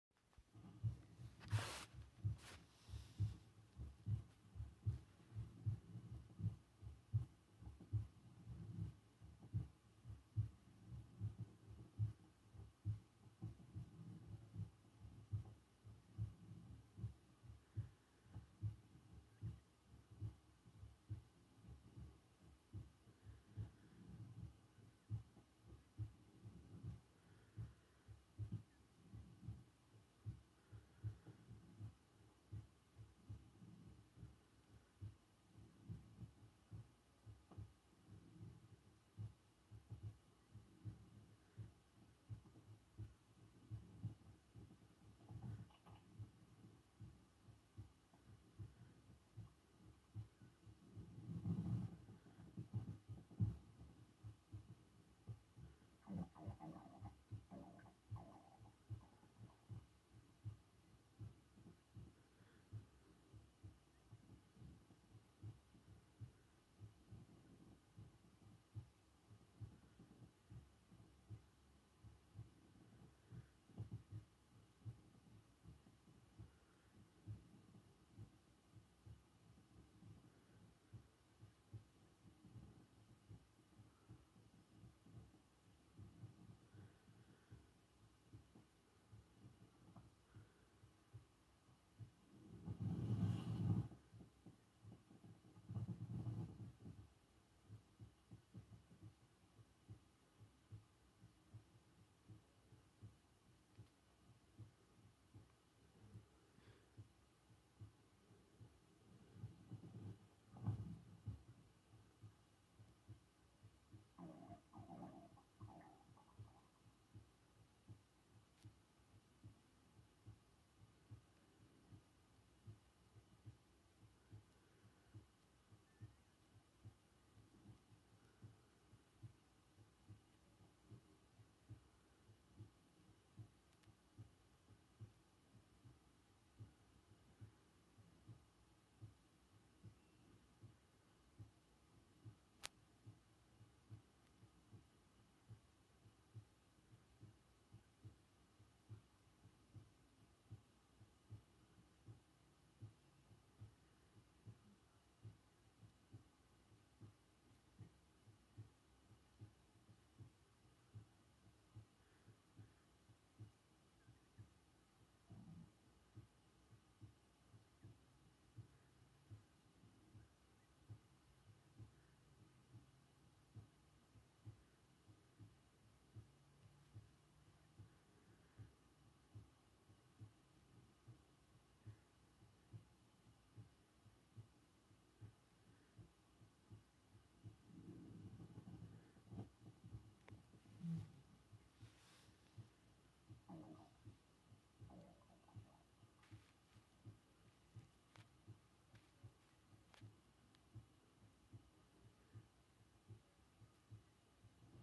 birthday feast aftermath 🐰💖💖🥰🥰
Aero the bunny is stuffed for eating too much cake and he's going to take a nap 😴